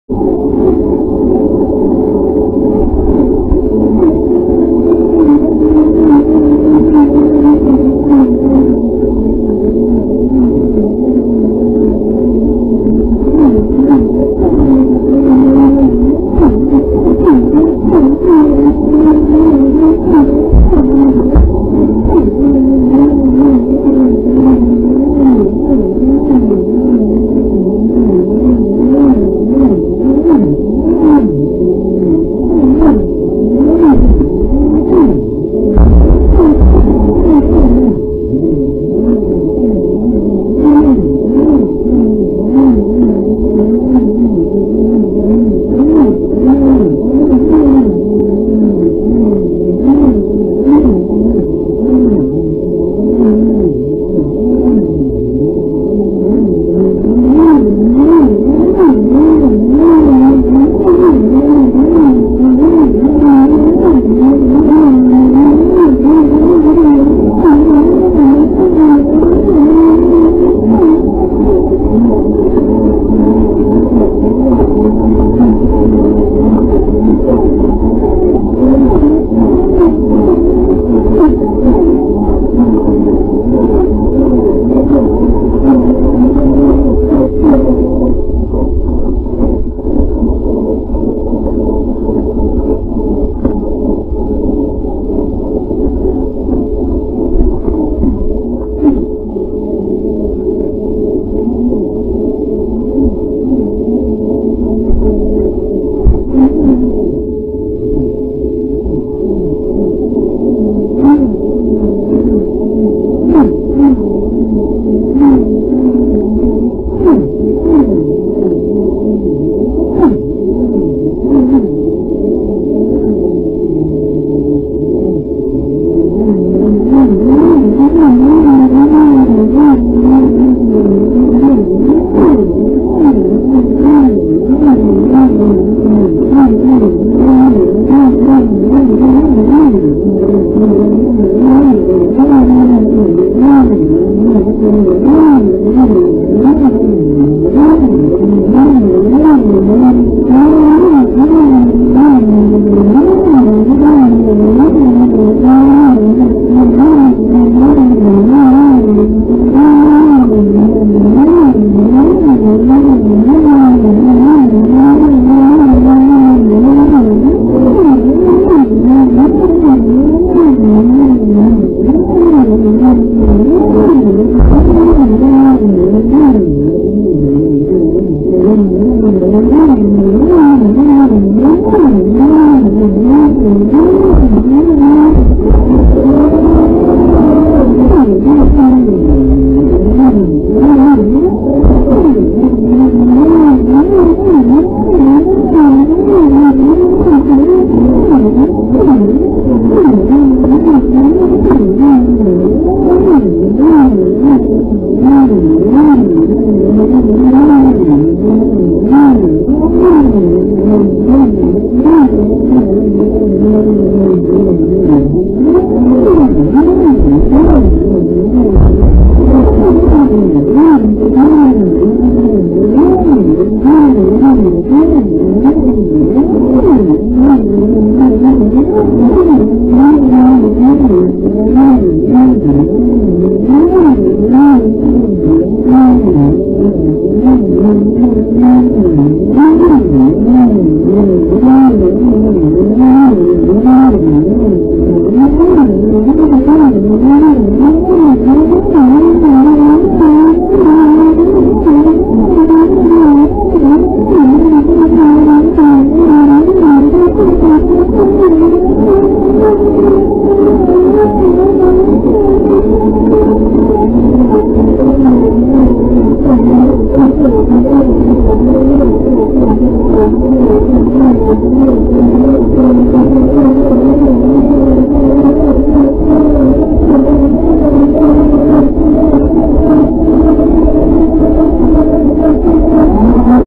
Cathedral Park River Splashing - Traxxas Slash 2wd w/ProLine shocks/swampers.
Now smells like river water.